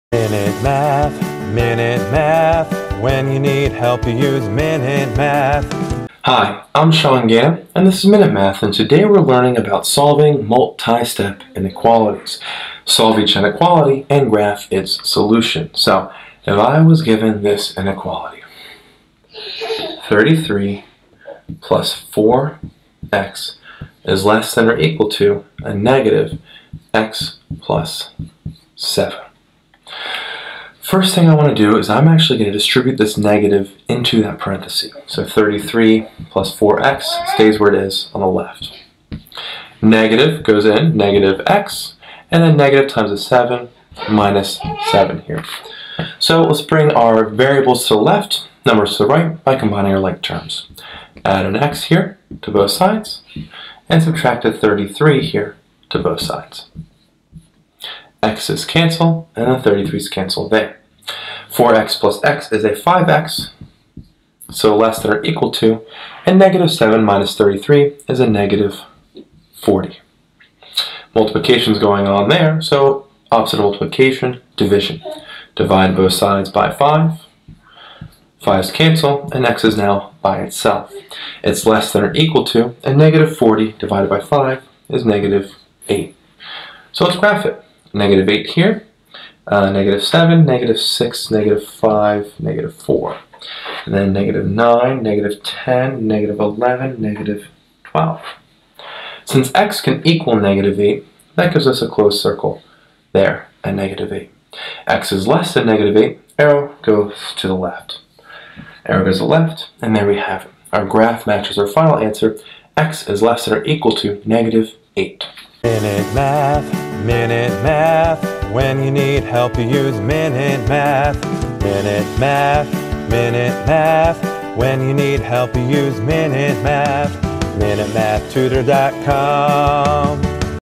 Solve and Graph 33+4x≤-(x+7)
In this math video lesson on Solving Multi-Step Inequalities, I solve the inequality 33+4x less than or equal to -(x+7) and graph its solution.